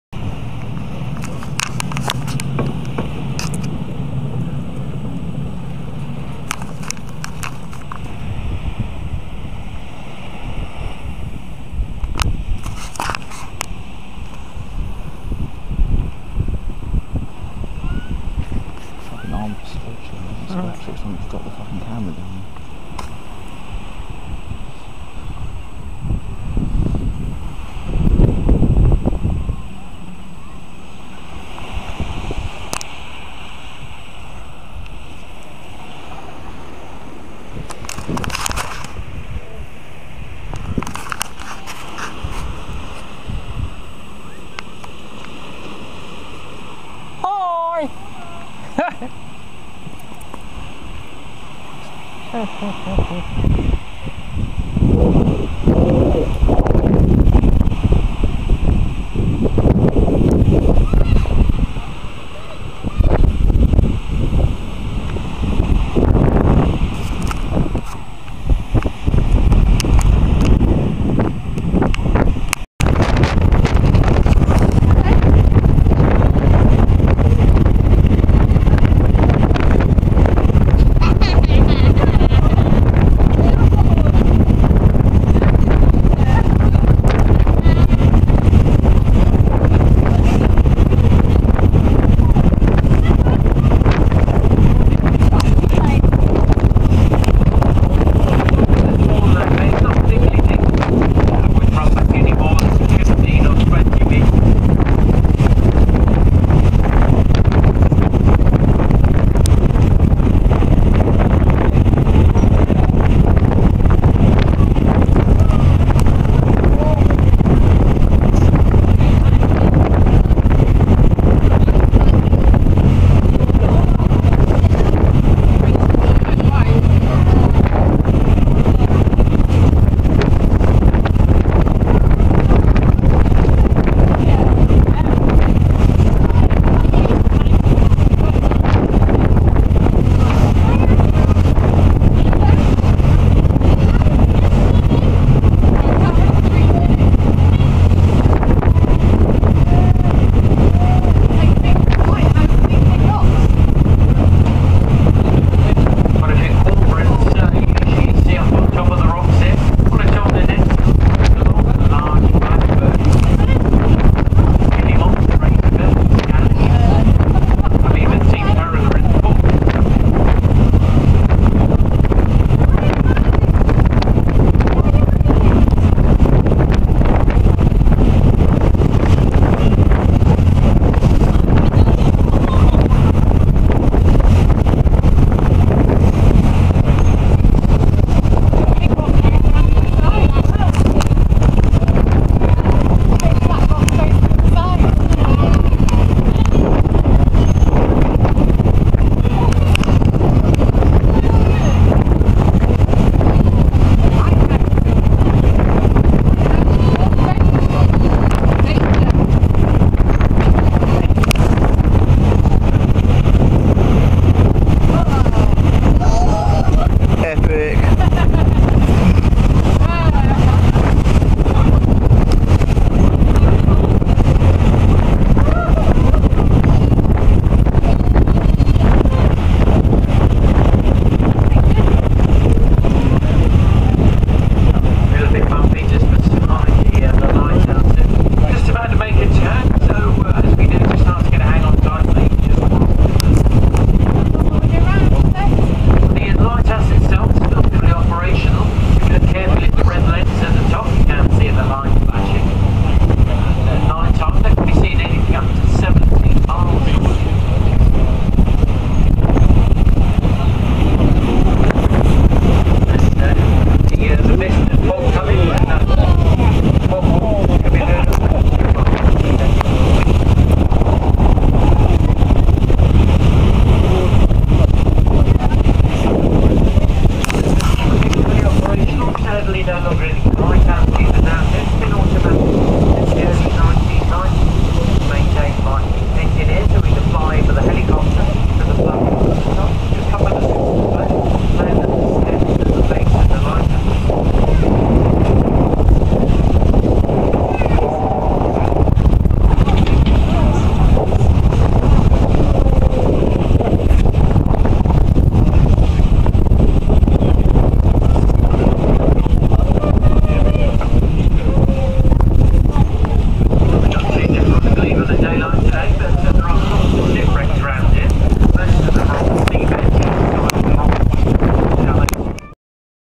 The Needles
Chairlift down the cliffs at Alum Bay and close up of the needles rock formation via boat trip.